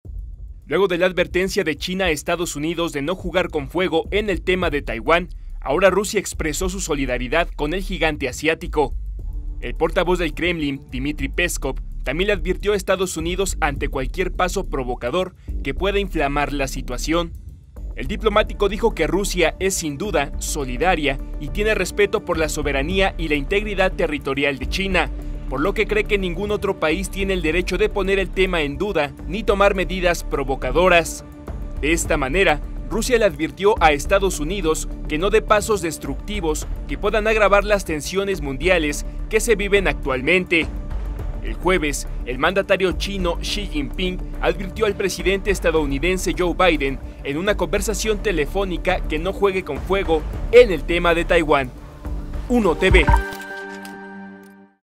“No provoques a China”: ahora Rusia lanza advertencia a Estados Unidos
Luego de la advertencia de China a Estados Unidos de “no jugar con fuego” en el tema de Taiwán, ahora Rusia expresó su “solidaridad” con el gigante asiático. El portavoz del Kremlin, Dmitri Peskov, también le advirtió a Estados Unidos ante cualquier paso “provocador” que pueda “inflamar” la situación.